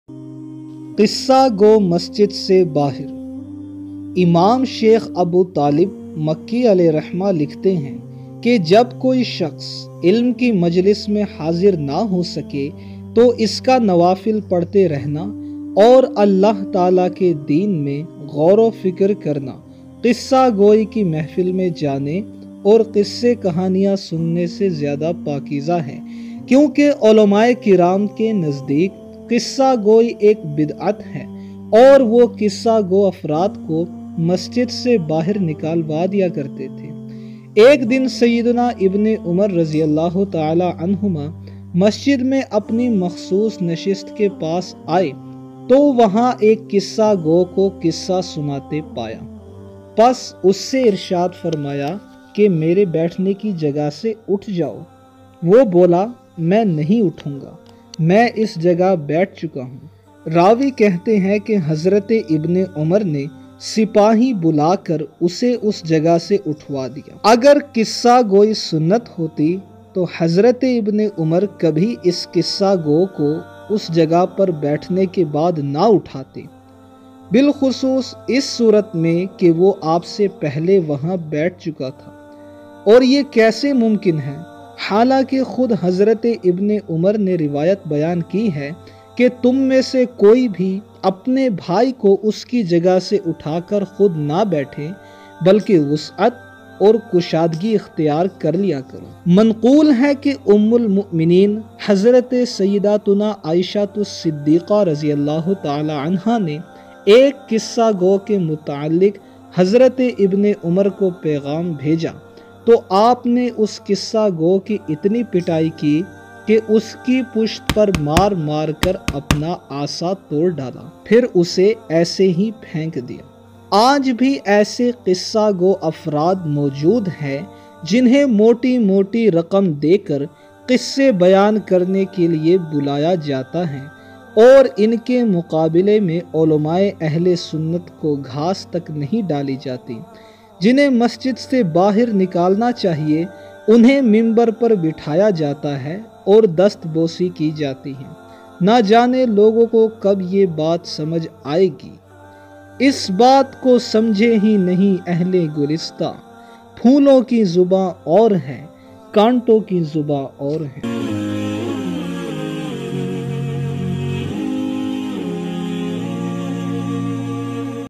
Masjid Se Bahar Kar Diya | Qissa Go Muqarrir | Abde Mustafa Official | PURE SUNNI INTERNATIONAL
Qissa Go Masjid Se Baahar

Imam Shaykh Abu Talib Makki Alaihi Rahma (M386H) Likhte Hain Ke Jab Koi Shakhs Ilm Ki Majlis Mein Haazir Na Ho Sake To Us Ka Nawafil Padhte Rehna Aur Allah Ta'ala Ke Deen Mein Ghauro Fikr Karna, Qissa Goyi Ki Mehfil Mein Jaane Aur Qisse Kahaniya Sunne Se Zyada Pakeeza Hai Kyunki Ulama -e- Kiraam Ke Nazdeek Qissa Goyi Ek Bidd'at Hai Aur Wo Qissa Go Afraad Ko Masjid Se Baahar Nikal Diya Karte The, Chunanche :

Ek Din Sayyiduna Ibne Umar Radiallaho Ta'ala Anhuma Masjid Mein Apni Makhsoos Nashist Ke Paas Aaye To Wahan Ek Qissa Go Ko Qissa Sunate Paaya, Pas Us Se Irshad Farmaya : Mere Baithne Ki Jagah Se Uth Jaao
Wo Bola : Main Nahin Uthunga, Main Is Jagah Baith Chuka Hoon
Raawi Kehte Hain Ke Hazrate Ibne Umar Ne Sipahi Bula Kar Use Us Jagah Se Uthwa Diya!

Agar Qissa Goyi Sunnat Hoti To Hazrate Ibne Umar Kabhi Us Qissa Go Ko Us Jagah Par Baithne Ke Baad Na Uthate Bil Khusoos Is Soorat Mein Ke Wo Aap Se Pehle Wahan Baith Chuka Tha Aur Ye Kaise Mumkin Hai Halanki Khud Hazrate Ibne Umar Ne Riwayat Bayaan Ki Hai Ke "Tum Mein Se Koi Bhi Apne Bhai Ko Us Ki Jagah Se Utha Kar Khud Na Baithe Balki Wus'at Aur Kushadgi Ikhteyar Kar Liya Karo"
(صحیح مسلم، کتاب السلام)

Manqool Hai Ke Ummul Momineen Hazrate Ayesha Siddiqa Radiallaho Ta'ala Anha Ne Ek Qissa Go Ke Mutalliq Hazrate Ibne Umar Ko Paigham Bheja To Aap Ne Us Qissa Go Ki Itni Pitayi Ki Ke Us Ki Pusht Par Maar Maar Kar Apna Asaa Tod Daala, Phir Use Aise Hi Phenk Diya

(قوت القلوب، اردو، ج1، ص336، 337، ملخصاً)

Aaj Bhi Aise Qissa Go Afraad Maujood Hain Jinhein Moti Moti Raqam De Kar Qisse Bayaan Karne Ke Liye Bulaya Jaata Hai Aur In Ke Muqable Mein Ulama -e- Ahle Sunnat Ko Ghaas Tak Nahin Daali Jaati
Jinhein Masjid Se Baahar Nikalna Chahiye Unhein Mimbar Par Bithaya Jaata Hai Aur Dast Bosi Ki Jaati Hai, Na Jaane Logon Ko Kab Ye Baat Samajh Aayegi

Is Baat Ko Samjhe Hi Nahin Ahle Gulista
Phoolo Ki Zubaa Aur Hai Kaanto Ki Zubaa Aur

Abde Mustafa